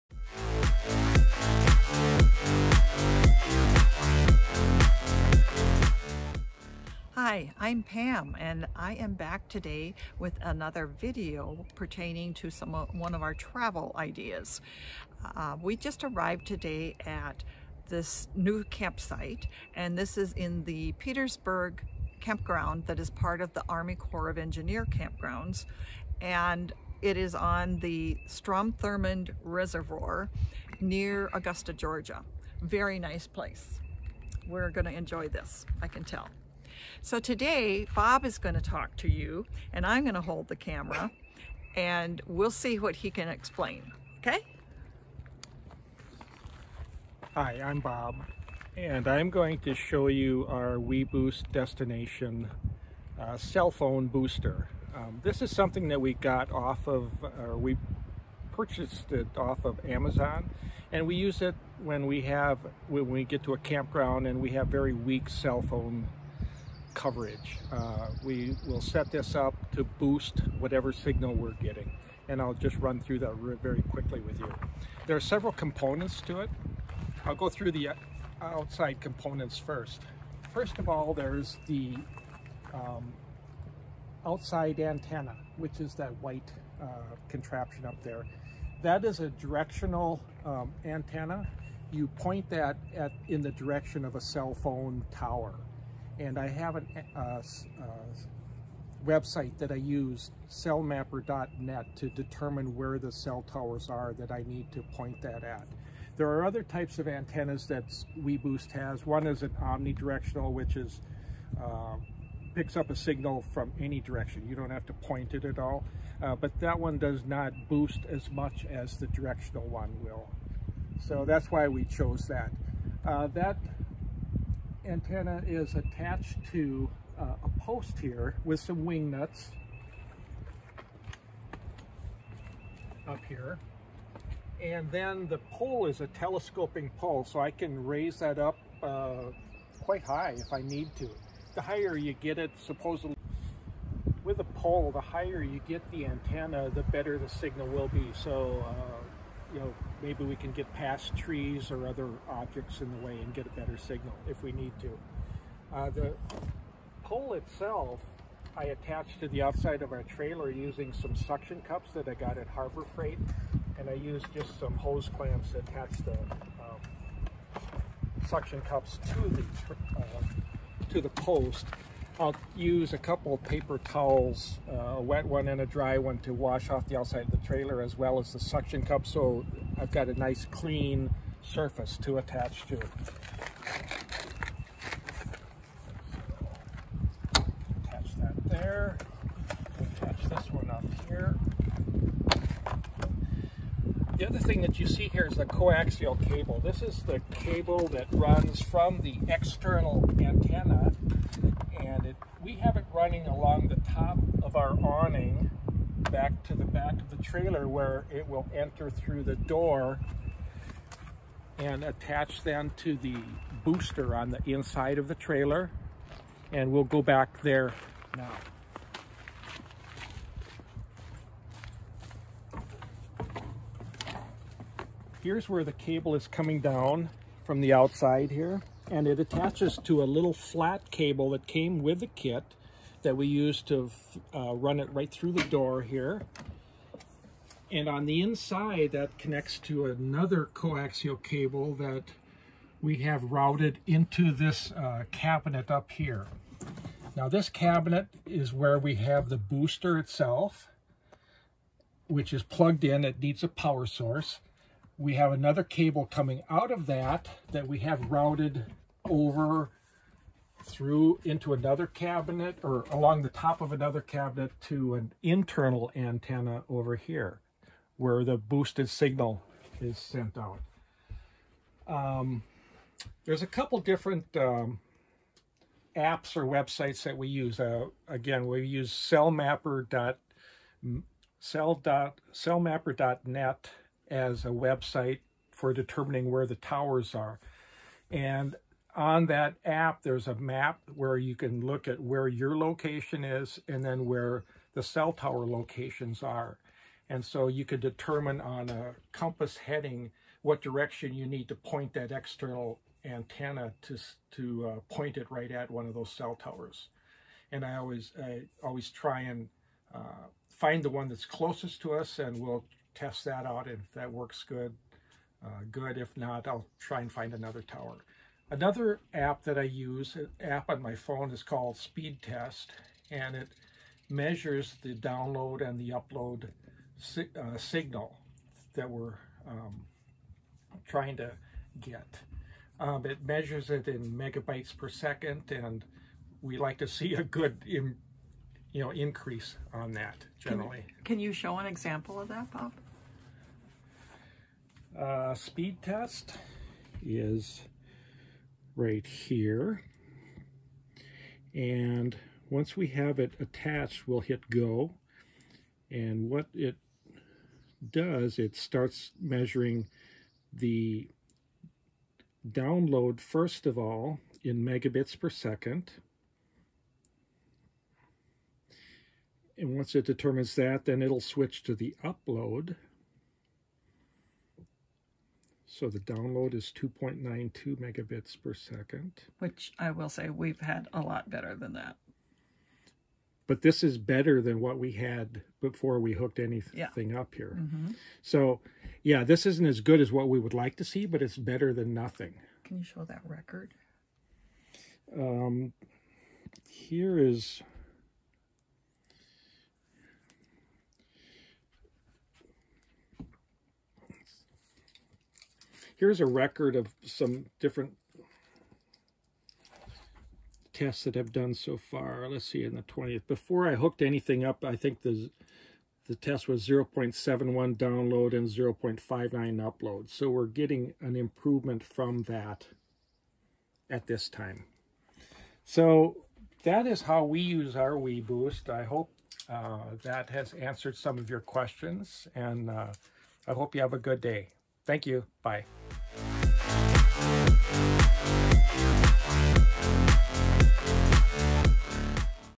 #17 – Searching For A Signal - Using a weBoost cell phone booster
We’ve been to many locations during our travels that have had a less than robust cellular signal. We don’t consider a cell signal a necessity as we don’t have a business or job that requires it. But for a lot of other reasons, especially safety, there is a comfort in the fact that we can make a phone call easily or reach out when we need to.

After a lot of research, we decided to use an additional antenna to capture and boost any cell signal we can find. The brand we are using is WeBoost and we have the RV Destination model.

Booster kit came with everything we needed except for the suction cups to mount it to the trailer.  Bob purchased ours at Harbor Freight.

I used some little command hooks, both inside and out, to get the wire cable to lay neatly around the cabinets.

Finally, make sure you have a step stool to reach everything safely.

Filmed using:
• iPhone SE
• Joby Gorillapod (Amazon)
• KODAK PhotoGear 62" Lightweight Tripod